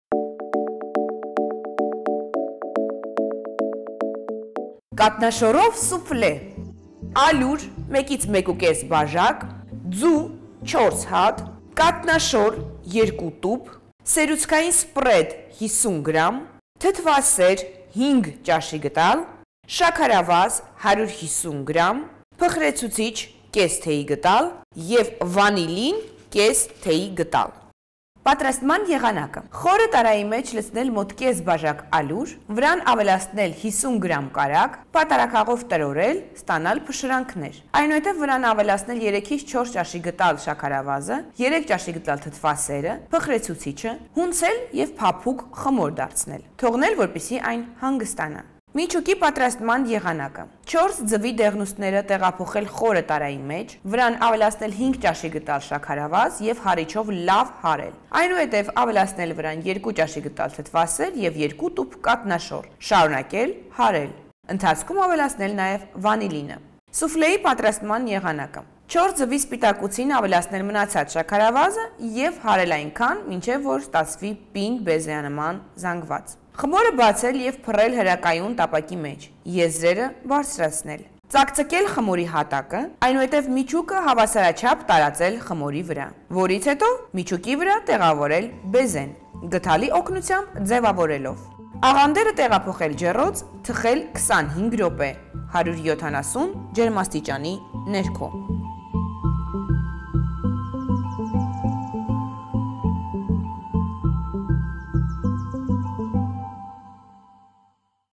Ի՞նչ եփել այսօր - Կաթնաշոռով սուֆլե
Բաղադրությունը՝
Ալյուր - 1-1.5 բաժակ
Ձու - 4 հատ
Կաթնաշոռ - 2 տուփ
Սերուցքային սպրեդ - 50 գ
Թթվասեր - 5 ճ.գդ.
Շաքարավազ - 150 գ
Փխրեցուցիչ - 0,5 թ.գդ.
Վանիլին - 0,5 թ.գդ.

Բարի ախորժակ